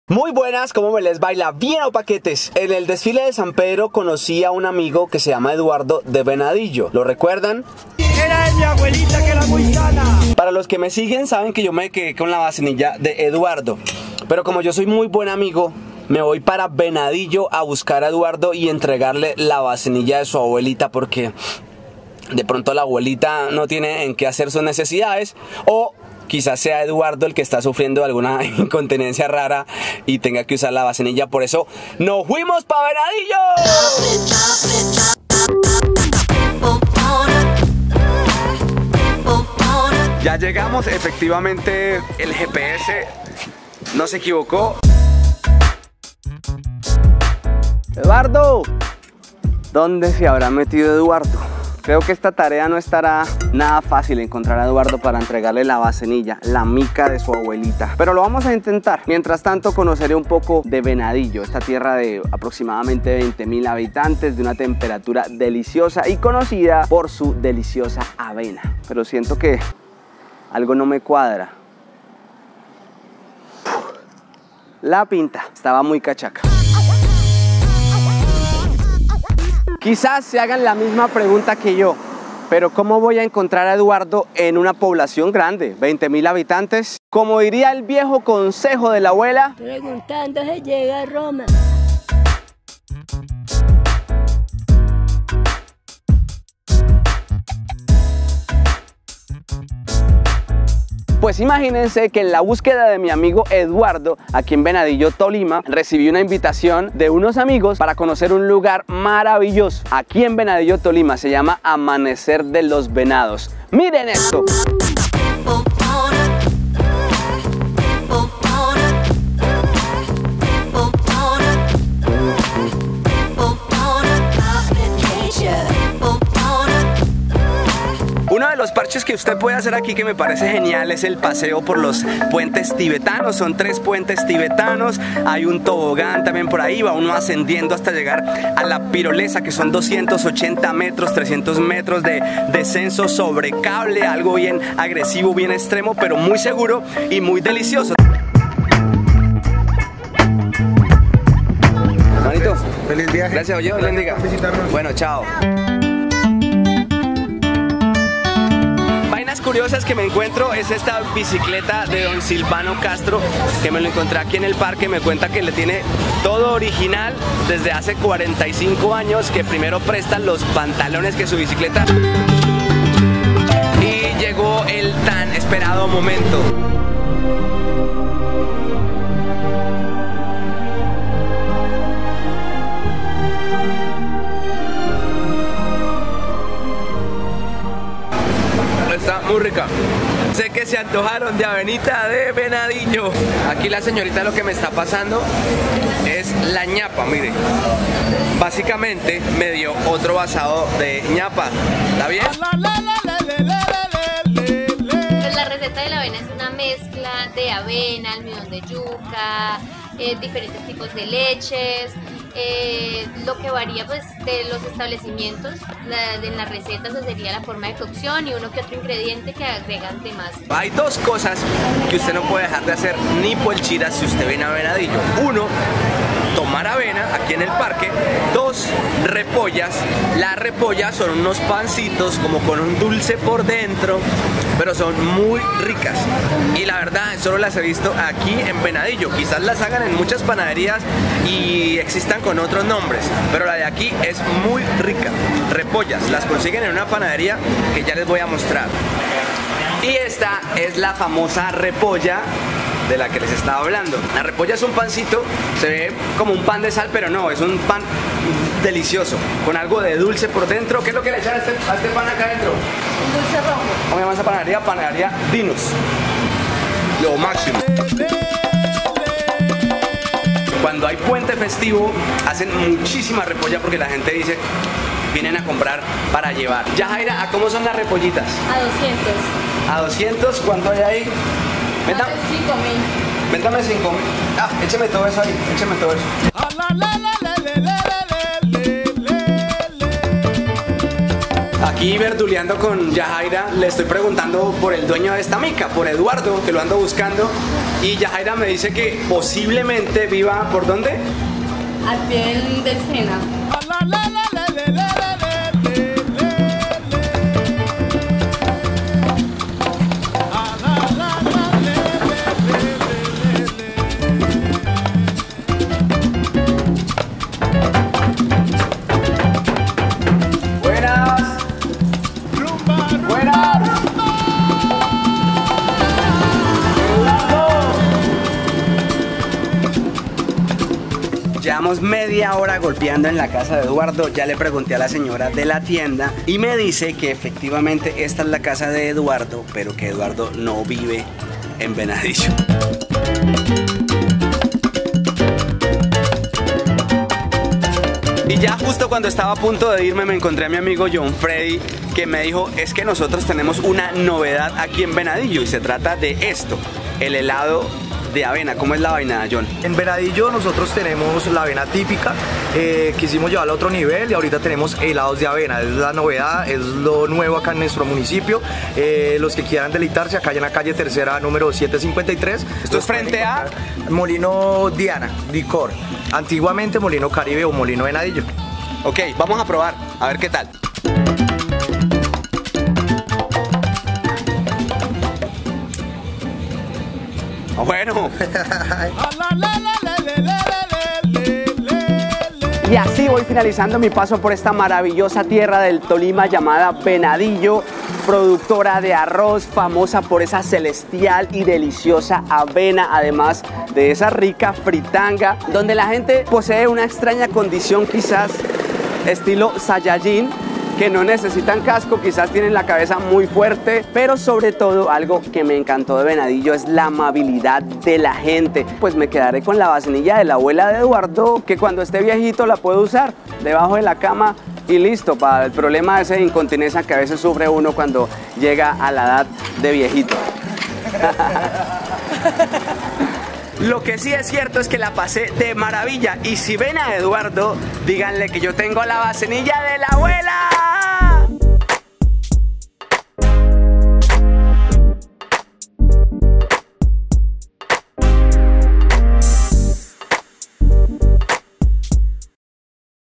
AVENA DE VENADILLO (LA MEJOR DEL MUNDO)
FUI A VISITAR EL MUNICIPIO DE VENADILLO TOLIMA
CONOCIDA POR LA AVENA ,FRITANGA Y POR SU GENTE AMABLE.